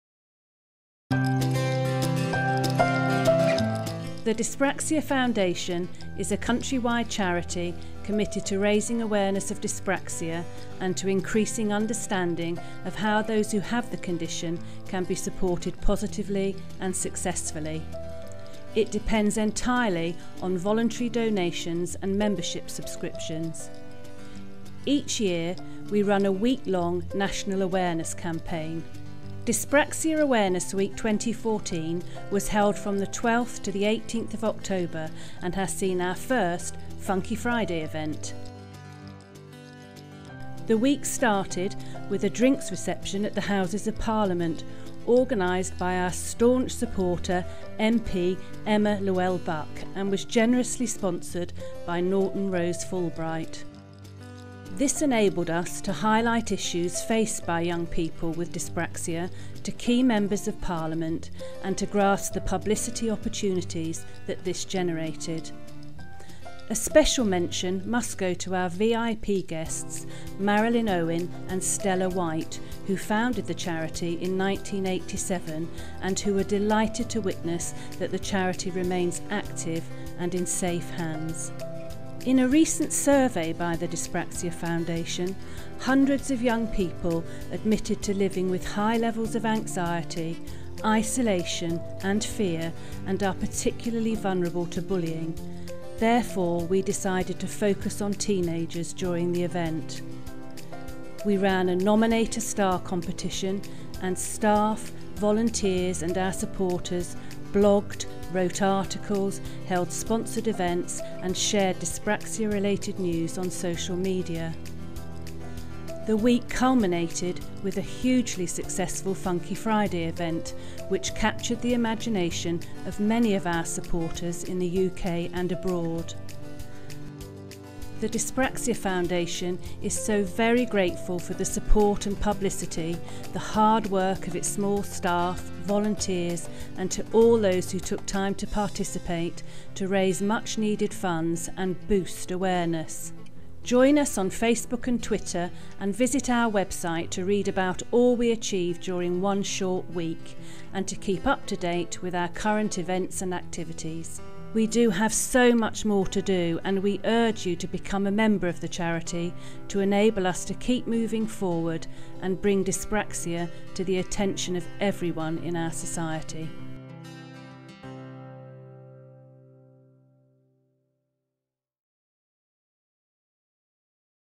Dyspraxia Awareness Week and Funky Friday 2014
Dyspraxia Awareness week 2014 was held from the 12th to 18th of October and has seen our first Funky Friday event. During the week we highlight issues faced by young people with dyspraxia to key members of parliament, taking advantage of the publicity opportunities that this generated.
The week culminated with a hugely successful Funky Friday Event which captured the imagination of many of our supporters in the UK and abroad.